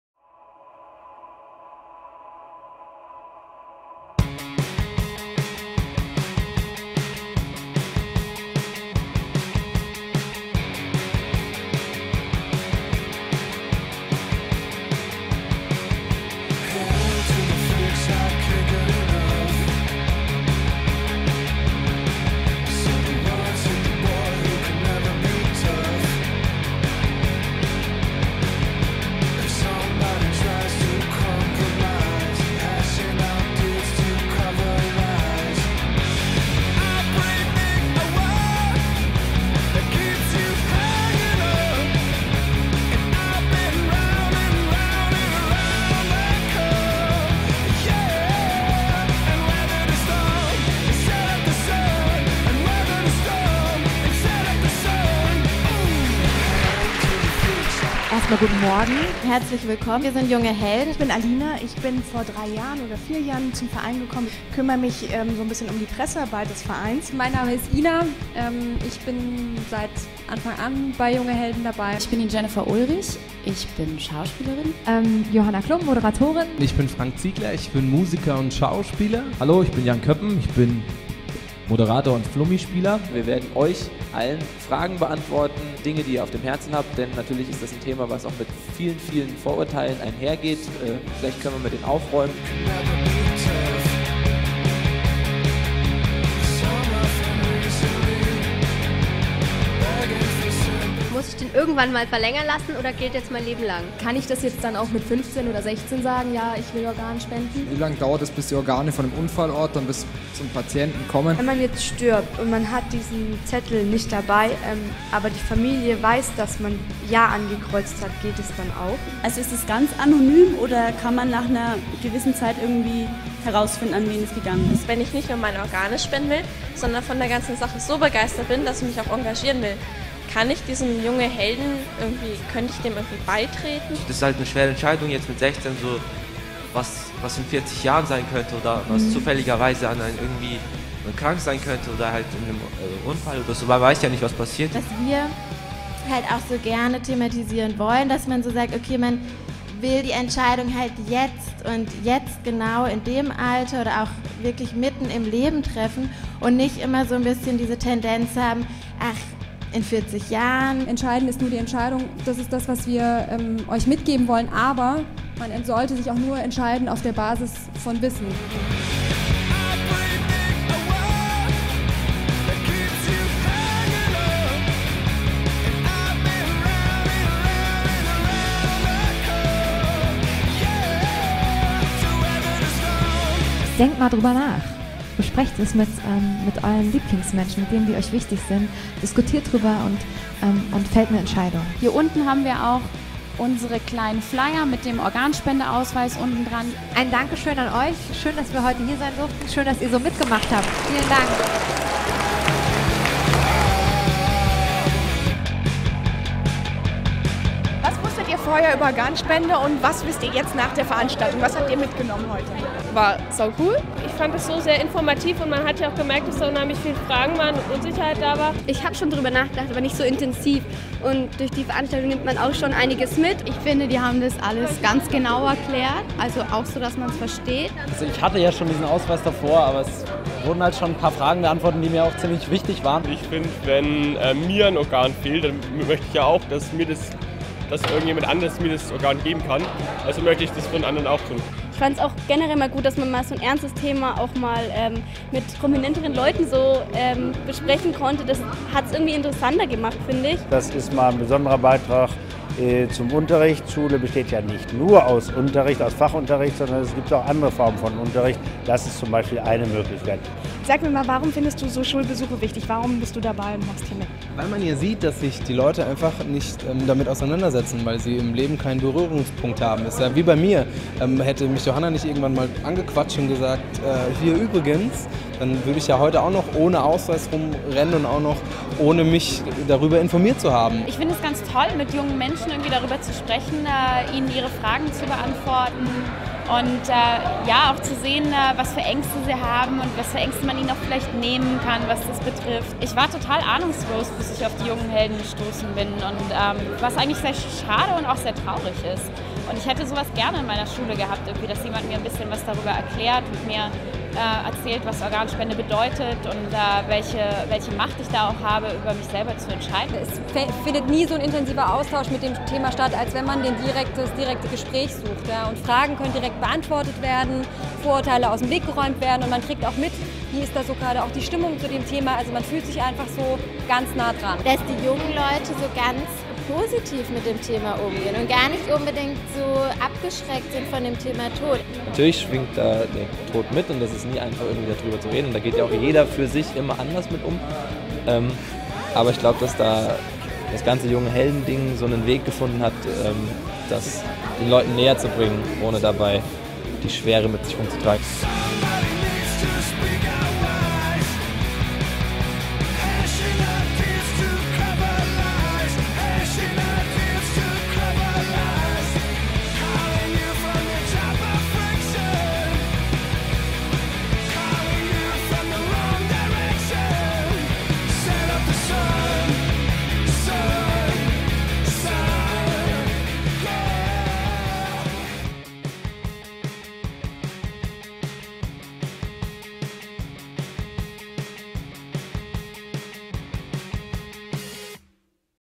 Junge Helden an Deiner Schule - Nürnberg
Gemeinsam mit Johanna Klum, Jan Köppen, Jennifer Ulrich und Frank Ziegler besuchte das Junge Helden-Team die Wilhelm-Löhe-Schule in Nürnberg, um mehrere Hundert Schüler in 1,5-stündigen Session über das Thema aufzuklären, Fragen zu beantworten, Vorurteile aus dem Weg zu räumen und natürlich auch das ein odere Autogramm zu schreiben.